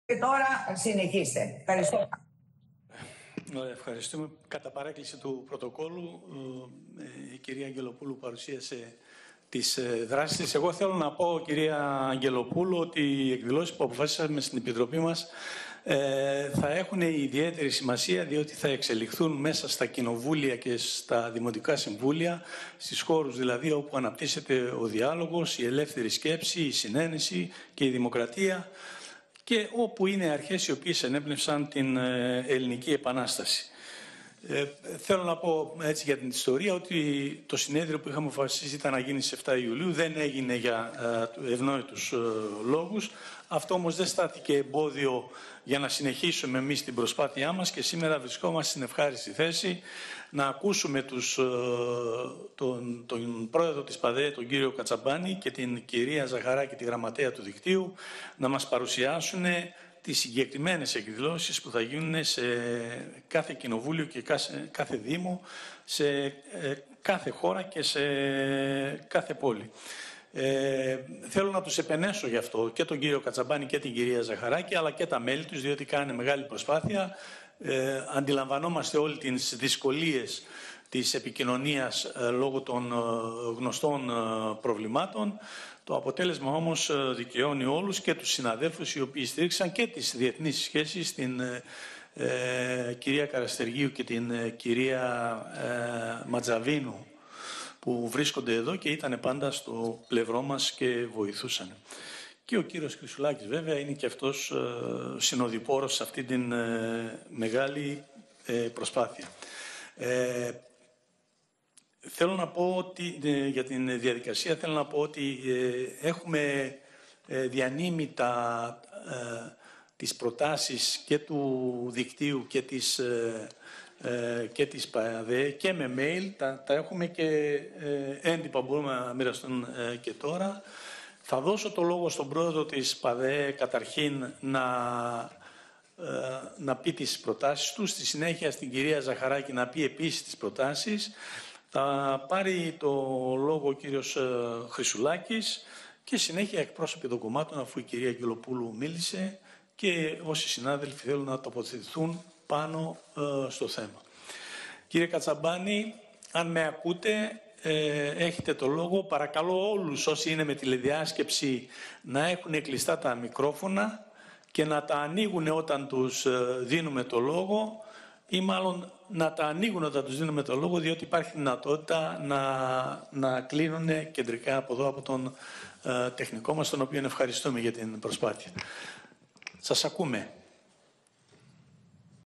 Συνεδρίαση της Επιτροπής Ελληνισμού της Διασποράς 2 14.10.2020
«Διοργάνωση εκδηλώσεων το 2021 από τα μέλη της Παγκόσμιας Διακοινοβουλευτικής Ένωσης Ελληνισμού (Πα.Δ.Ε.Ε) και τα μέλη του Δικτύου Ελλήνων Αιρετών Αυτοδιοίκησης της Ευρώπης για τα 200 χρόνια από την Ελληνική Επανάσταση»